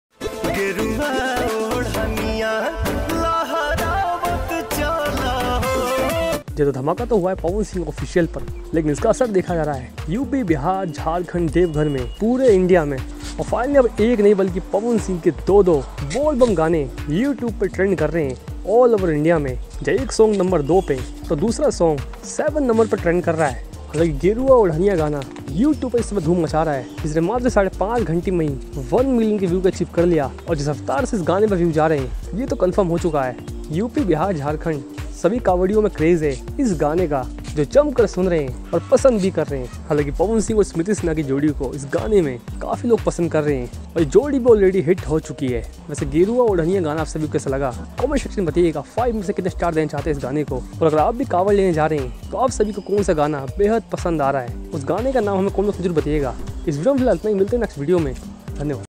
देखिये सावन में लाखों लोग झूम उठे पवन सिंह के गाने पर जमकर हुआ जाम रोड:चोक जाओगे
special thanks pawan singh